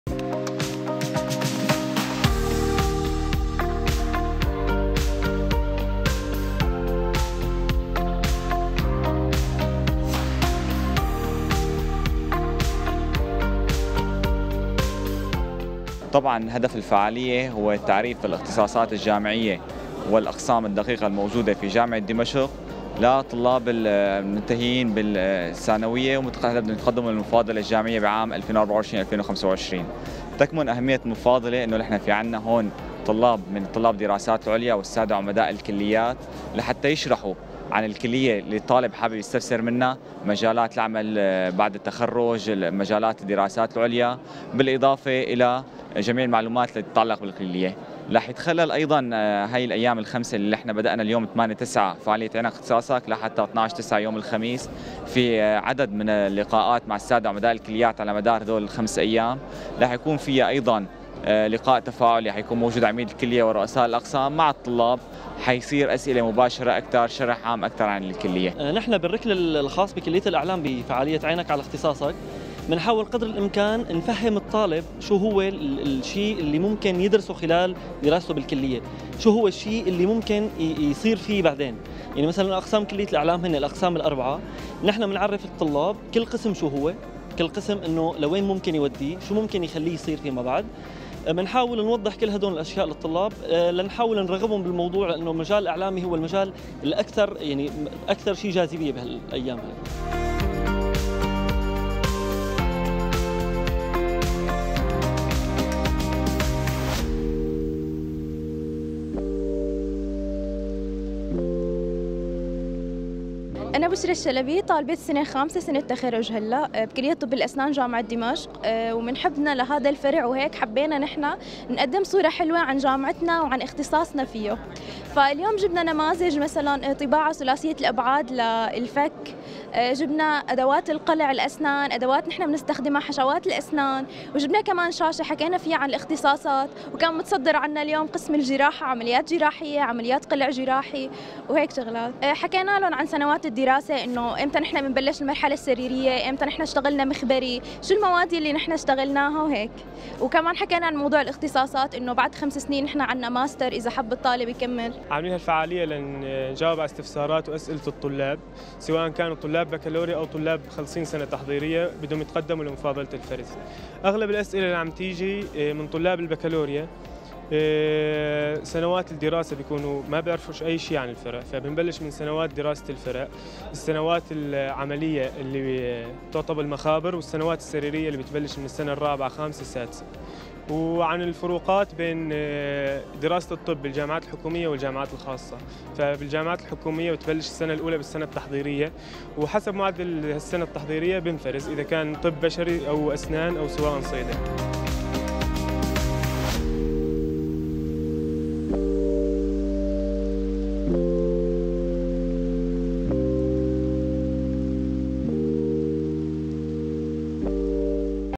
"عينك ع اختصاصك" لتعريف الطلبة الجُدد بالاختصاصات الجامعية
بهدف تعريف الطلبة الناجحين في الشهادة الثانوية بالاختصاصات الموجودة في الجامعات والمعاهد، وبالتزامن مع بدء التقدم للمفاضلة الجامعية، أطلق الاتحاد الوطني لطلبة سورية اليوم فعاليات النسخة الخامسة من معارض “عينك ع اختصاصك” التفاعلية في الجامعات.

sat 201
Fr:12206
Pol:Vertical
FEC:5/6
DVBS
Symbol Rate:27500